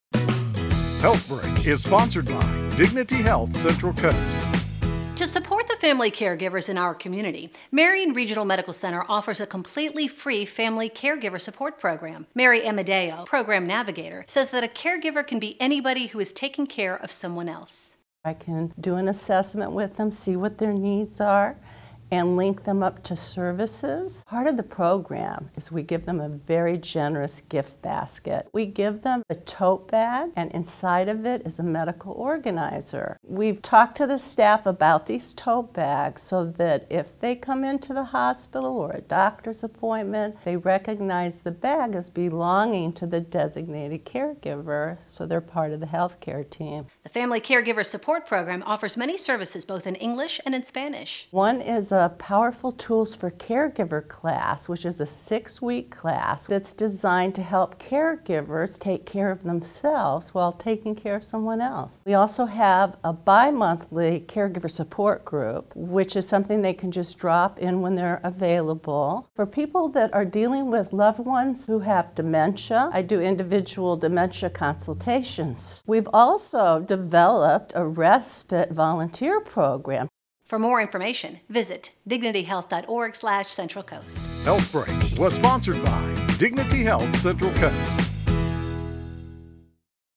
HealthBreak - Family Caregiving Support Program
Mary Amedeo, LCSW, Family Caregiver Navigator, Marian Regional Medical Center, discusses the Family Caregiving Support Program.